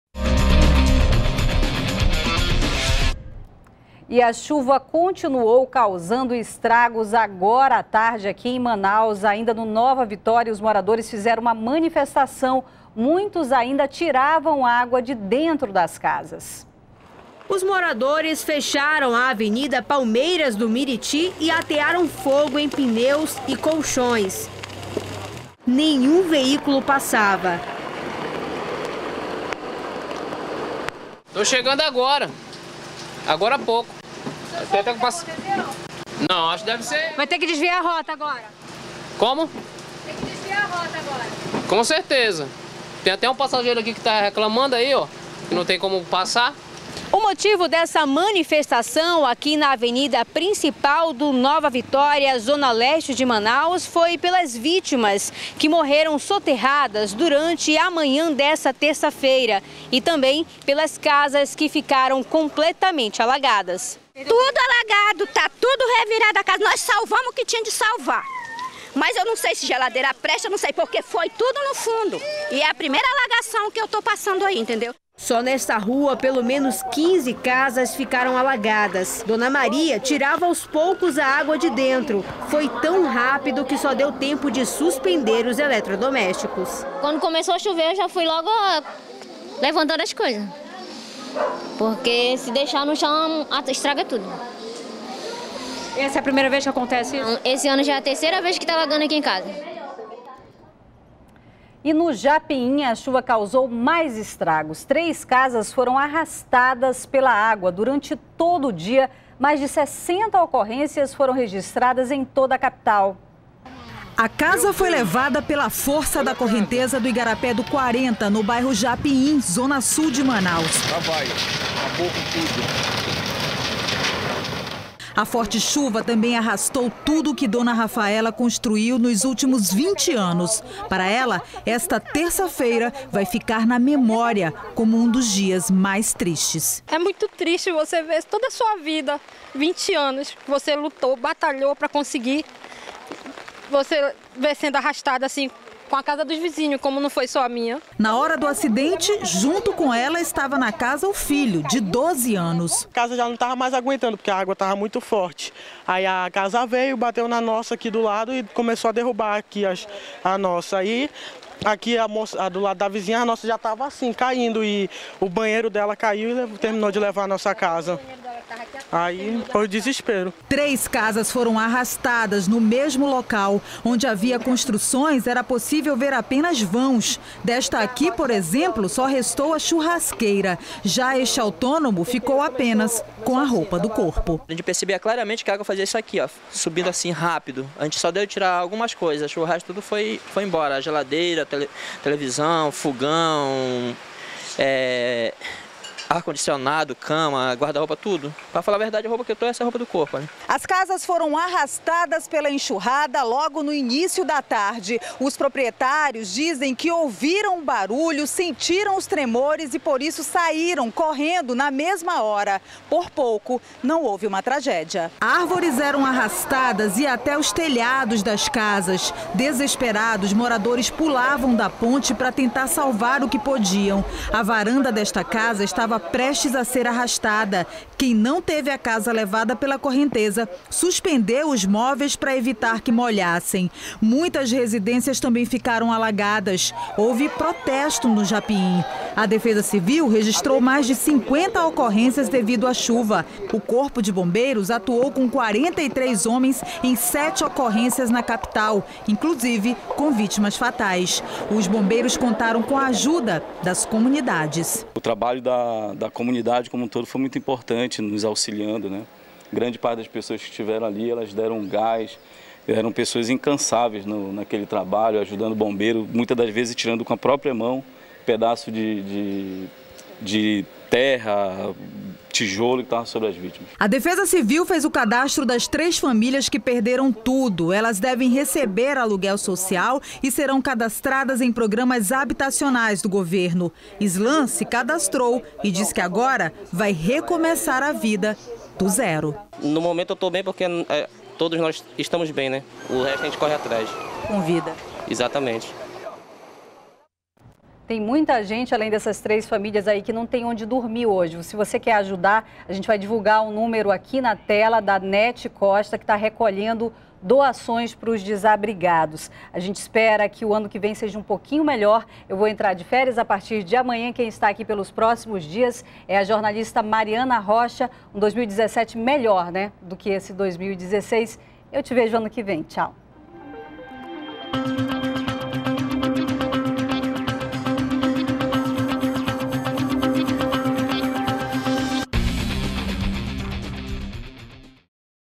03 casas foram arrastadas pela água, mais de 60 ocorrências foram registradas em Manaus
03º BLOCO JORNAL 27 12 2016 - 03 casas foram arrastadas pela água, mais de 60 ocorrências foram registradas em Manaus